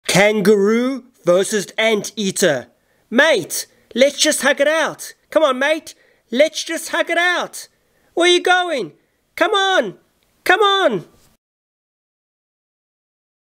Anteater Loves Hugs With Kangaroo With Epic Voice Over
Anteater love hugs with kangaroo with epic voice over
Who loves anteaters and kangaroos